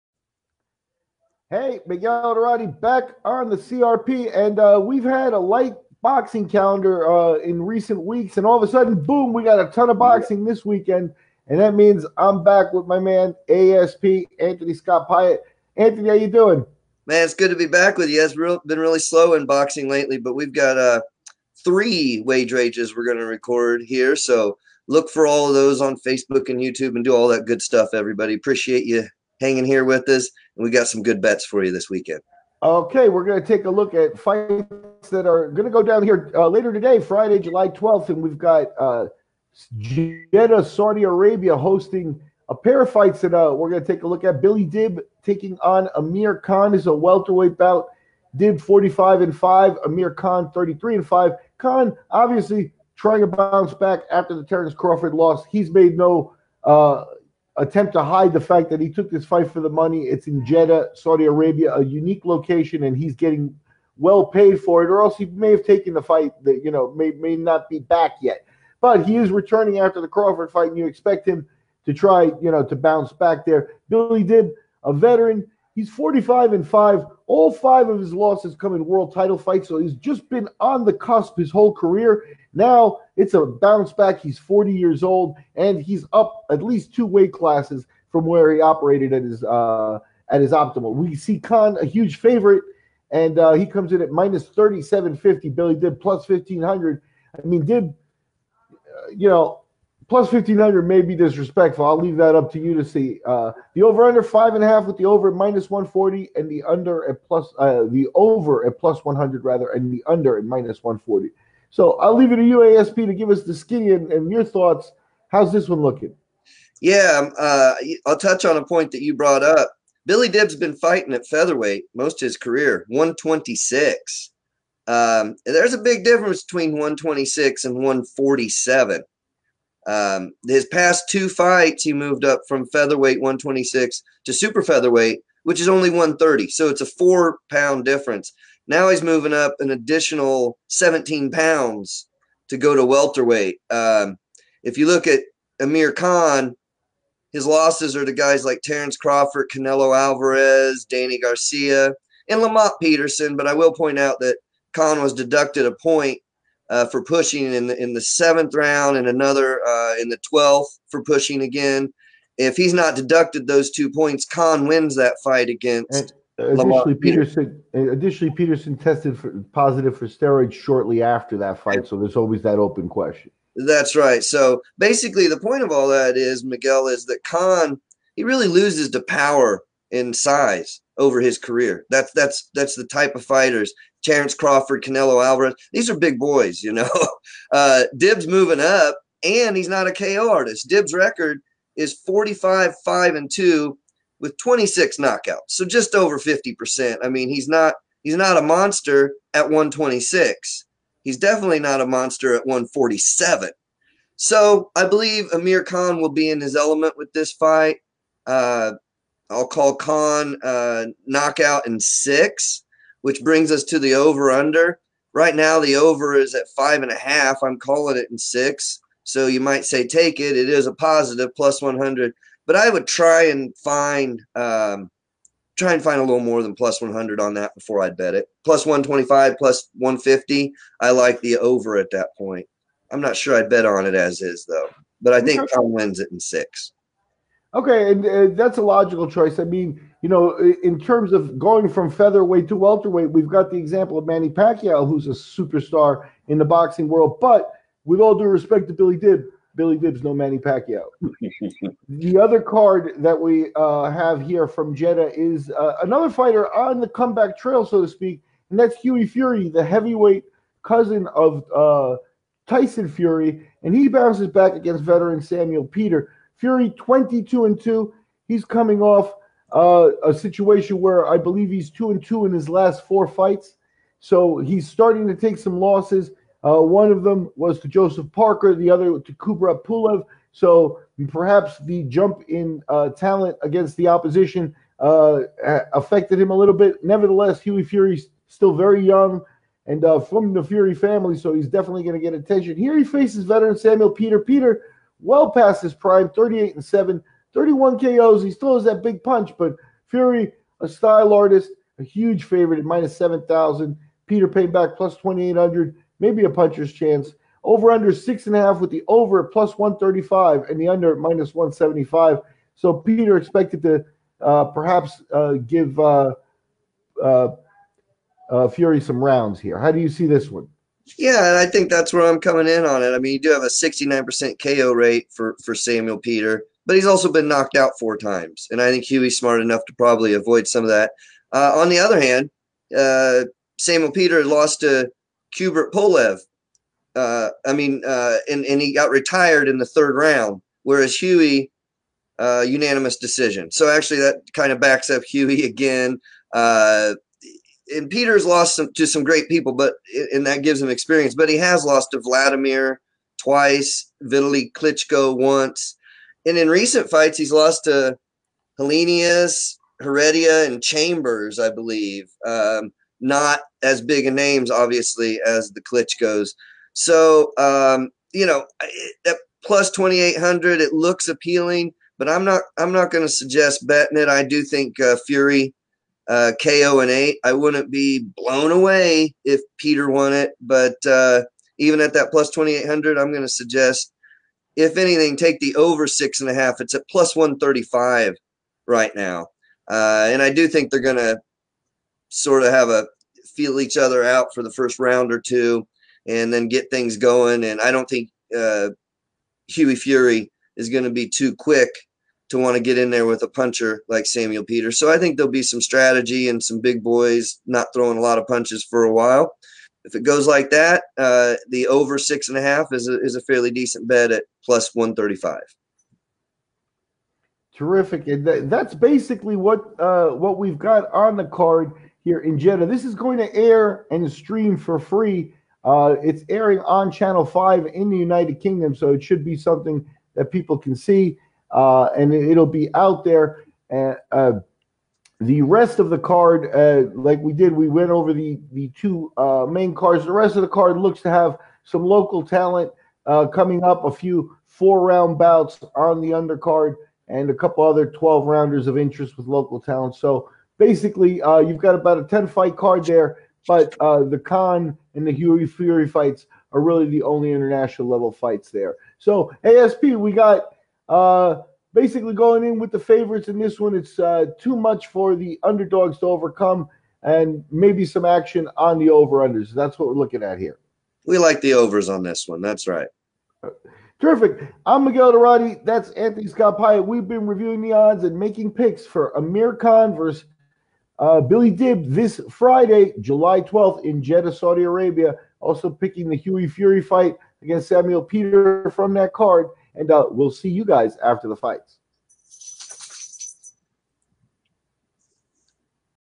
WAGE RAGE: Khan vs. Dib odds and picks
WAGE RAGE: Khan King Abdullah Sports City - Jeddah, Saudi Arabia - Friday, July 12, 2019 Welterweight 12 rounds - Billy Dib 45-5(2), 26KO +1500 Over 5½ +100 Listed (on boxrec.com) at featherweight 126, last couple at super featherweight 130, fighting welterwieght 147 Amir Khan 33-5(4), 20KO -3750 Under 5½ -140 L: Terrence Crawford, Canelo Alvarez, Danny Garcia, Lamont Peterson SD Khan deducted one point for pushing in the 7th round and another in the 12th for the same offence. Khan has lost to power and size over his career. Dib is moving up and is not a KO artist. So, I believe Amir Khan will be in his element. PREDICT: Khan, KO 6 BET: Over 5 ½ anything greater than +100 RESULT: Heavyweight 10 rounds - Samuel Peter 38-7(4), 31KO +2800 Over 6½ +135 69% KO rate, but also KO’d 4 times L: Kubrat Pulev RTD 3, Vladimir 2x’s, Vitali 1x, Helenius, Heredia, Chambers Hughie Fury 22-2, 12KO -7000 Under 6½ -175 L: Kubrat Pulev UD, Joseph Parker MD Only 50% KO rate, he’s not his brother, Tyson, but Hughie should pull off the win PREDICT: Fury, KO 8 BET: Over 6½ +135 AmirKhan BillyDib HughieFury SamuelPeter HeavyweightBoxing BoxingOdds BoxingNews CRP WageRage PiattPickvs. Dib odds and picks